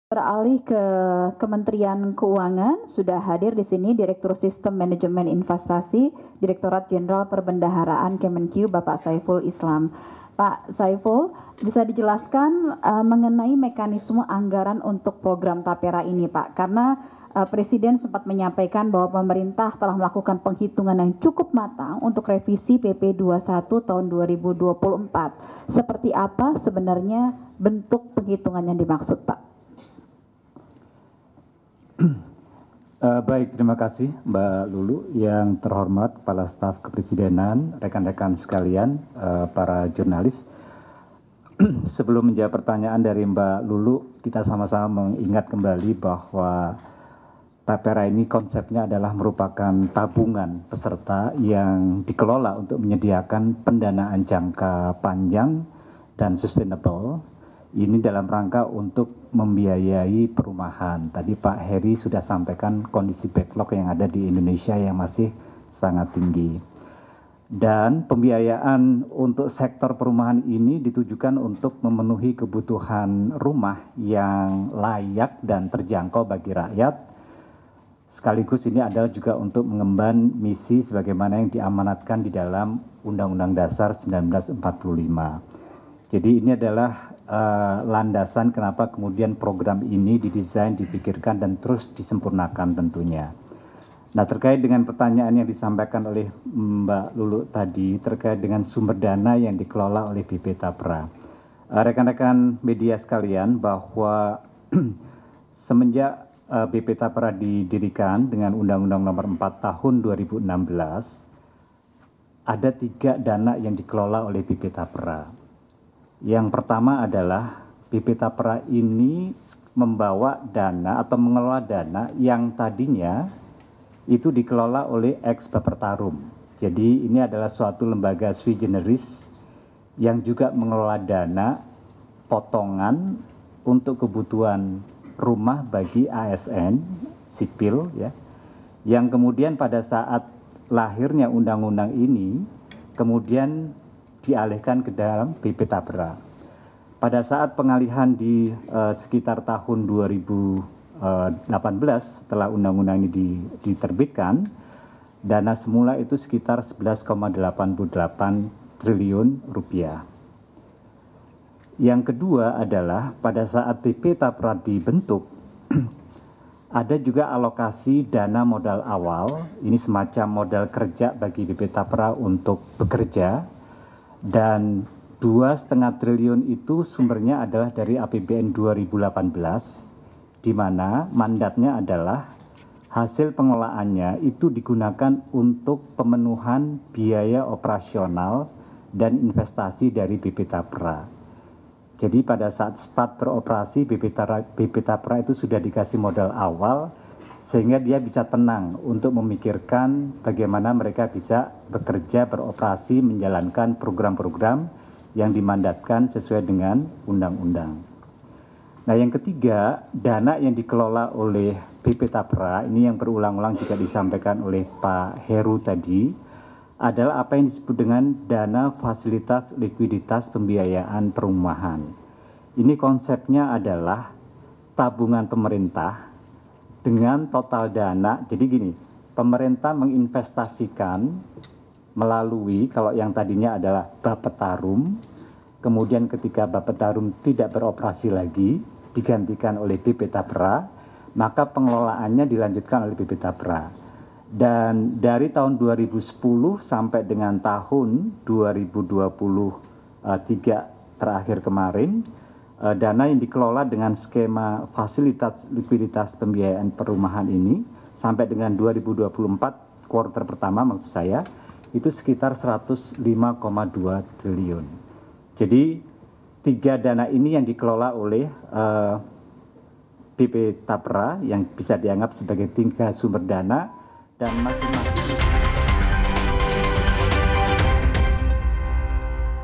Kemenkeu Jelaskan Sistem Pengelolaan Dana Tapera yang Dikumpulkan dari Gaji Pegawai
KOMPAS.TV - Direktur Sistem Manajemen Investasi Kemenkeu, Saiful Islam menjelaskan pengelolaan dana Tapera yang diambil dari gaji karyawan nantinya.

Saiful juga menjelaskan dana dari Bapertarum yang dialihkan ke Tapera.

Hari Jumat (31/05/2024) ini Badan Pengelola Tabungan Perumahan Rakyat atau BP Tapera, akan menggelar konferensi pers.